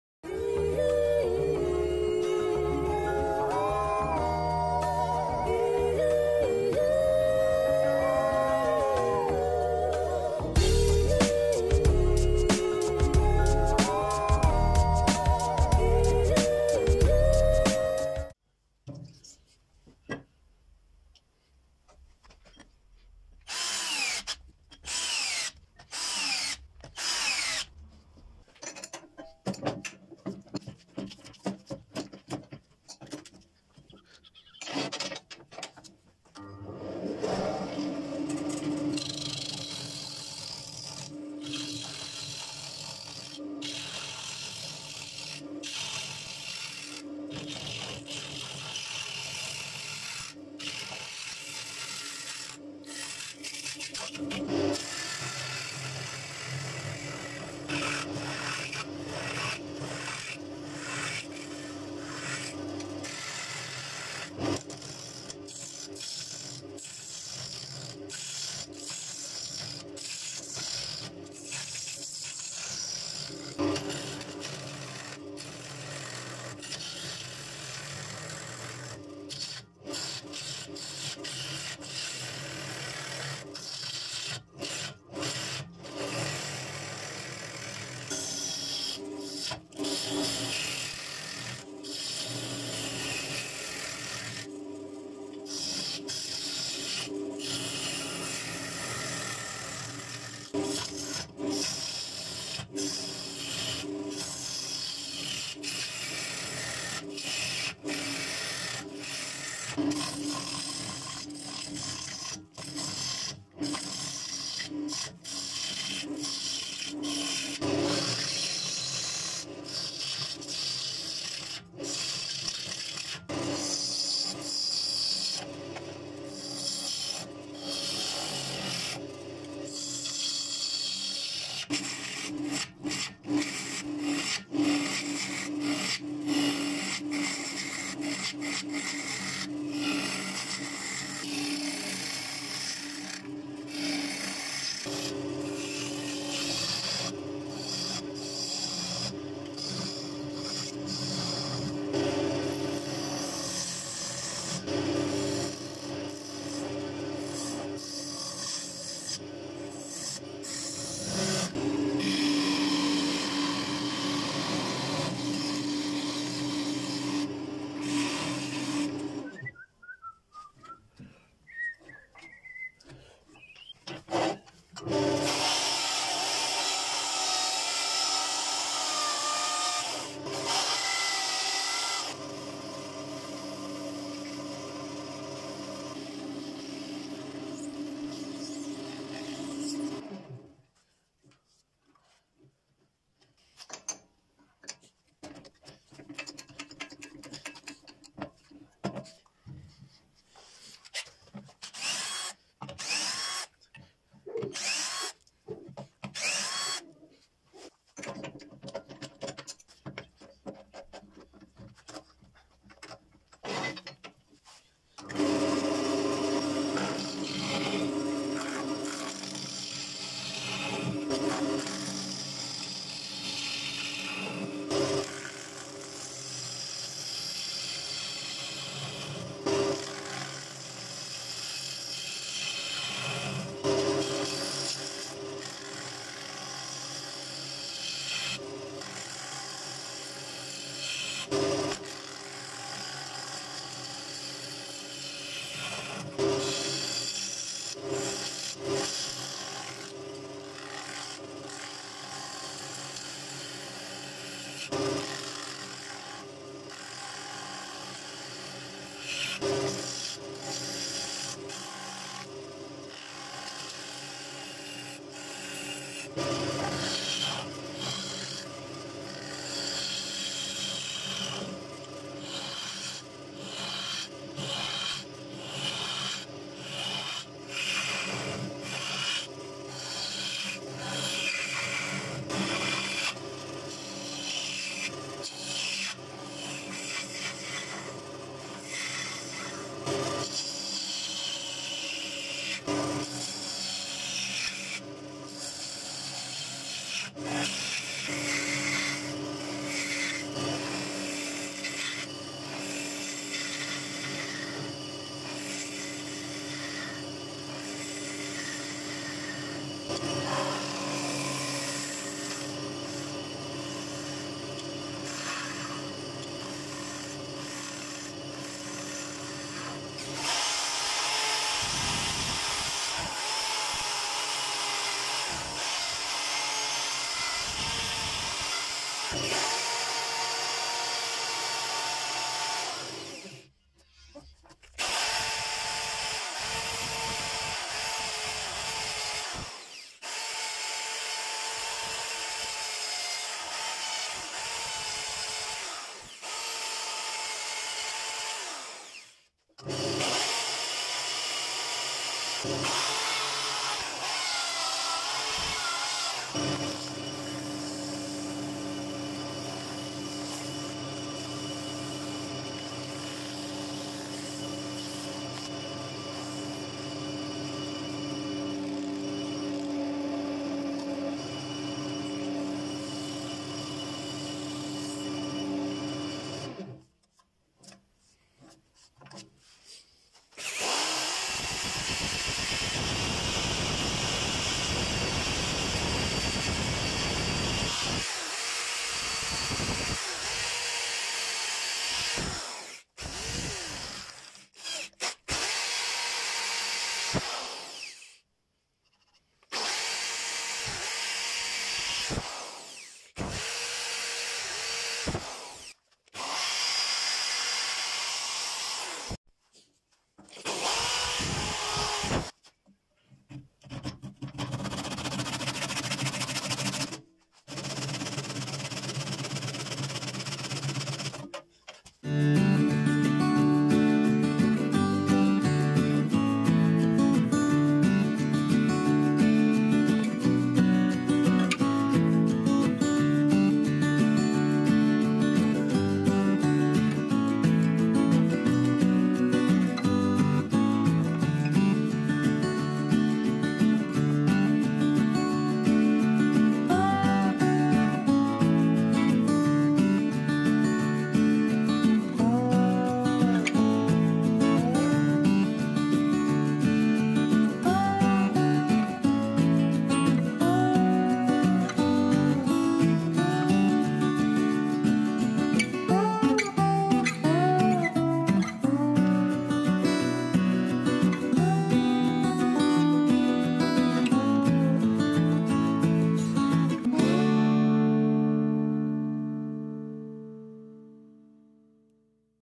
Woodturning - shard of pear tree trunk
Turning a piece of pear tree into an original bowl.

If you liked the video and want to see new videos, click Like and Bell.